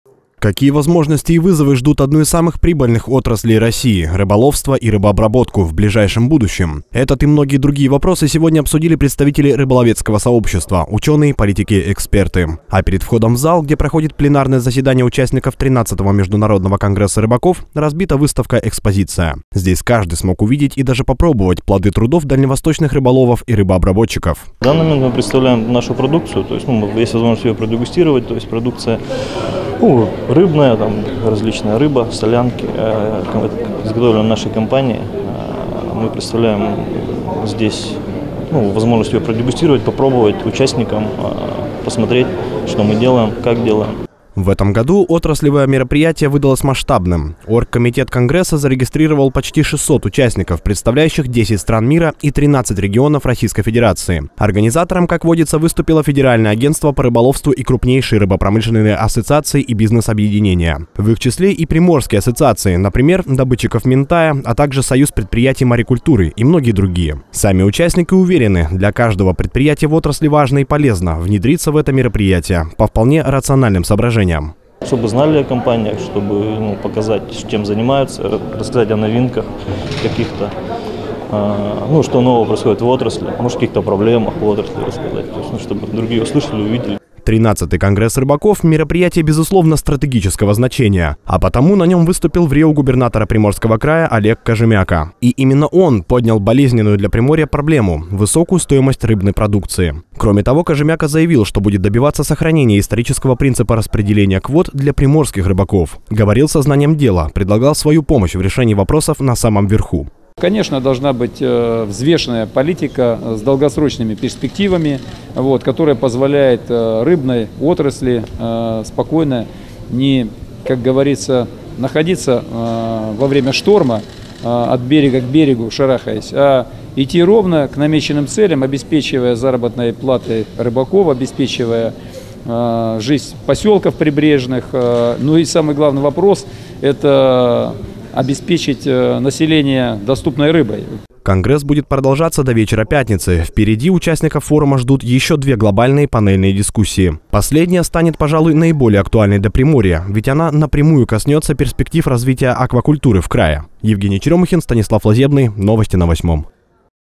КОНГРЕСС РЫБАКОВ
Во Владивостоке открылся  13-й  Международный конгресс рыбаков. Главная тема - «Рыбная отрасль – новый этап развития». В 2018 году отраслевой форум посвящен обсуждению тех возможностей и вызовов, которые ждут рыбную отрасль страны следующие 15 лет. Перед рыбацким  сообществом  выступил   врио Губернатора Приморского края Олег Кожемяко. На форуме он поднял и злободневную тему перевалки угля, отметив, что арендаторам причалов будут выставлены отдельные условия. Он  подчеркнул, что прежде чем перегружать уголь там, где живут люди, надо  проводить  общественные  слушания,   строить закрытые терминалы, заботиться о  безопасности  граждан.